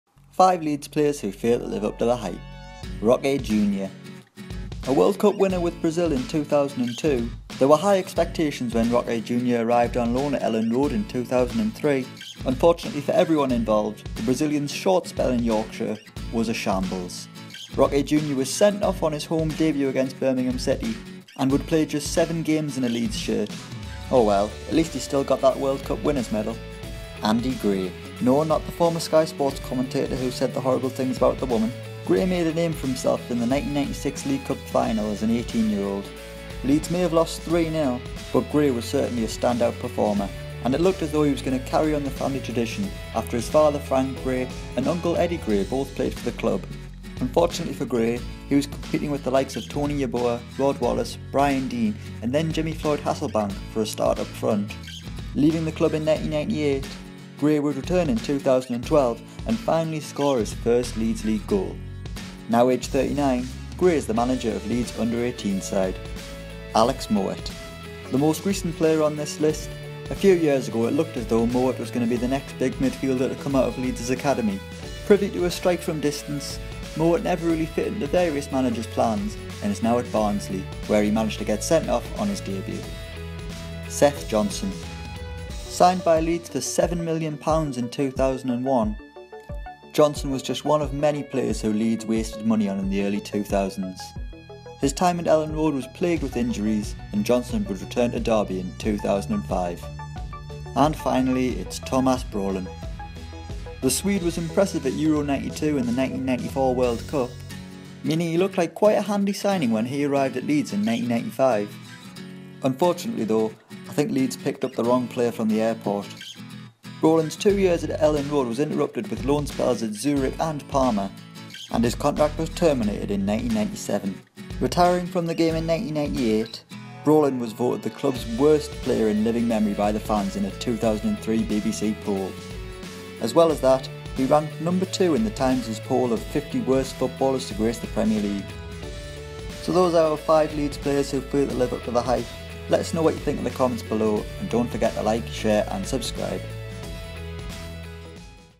5 Leeds United Players Who Failed To Live Up To The Hype
Leeds United fans are still wondering what happened to the real Tomas Brolin...